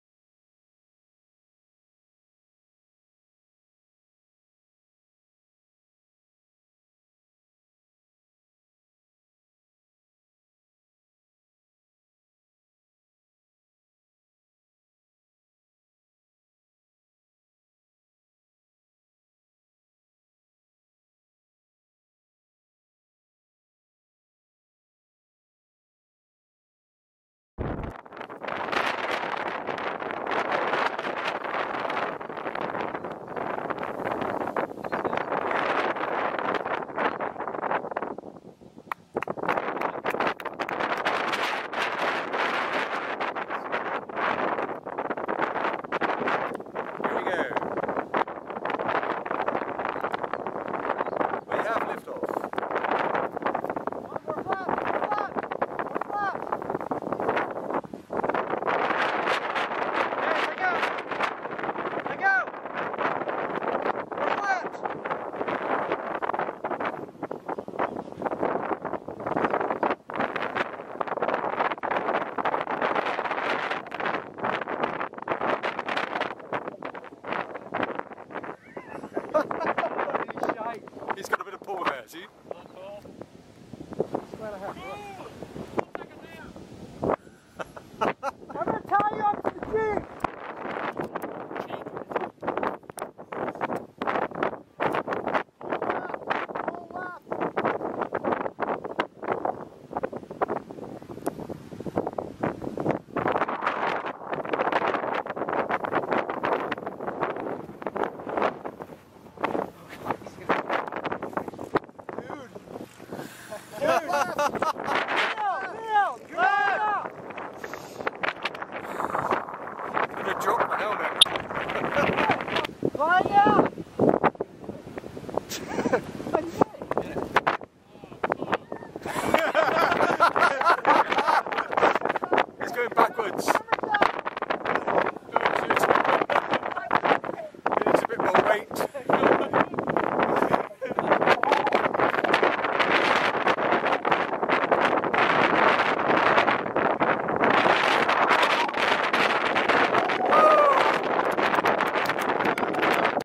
Neal Smith Tow Launch & Landing
A windy day stopped flying but we had 200 feet of rope and plenty of volunteers.  Neal is an experienced paramotor pilot so had no problems as you can see.  The rope was up at a very high angle so he must have gone to over 150 feet.  I put a few photos in but the sound is poor because of the wind - but at least that allowed us to fly just by applying some brake to get airborne!  BTW - the wing is an original MacPara Muse (1) and 30 Sqn Mtrs.